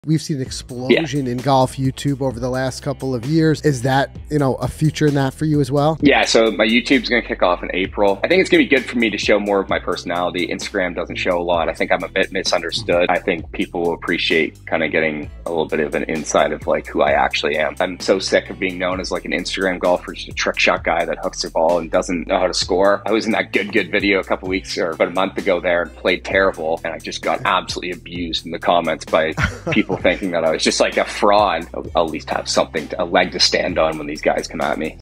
Can Mac Boucher Really Play Golf? 🤔
Mac Boucher shoots down rumors and announces the launch of his YouTube Channel.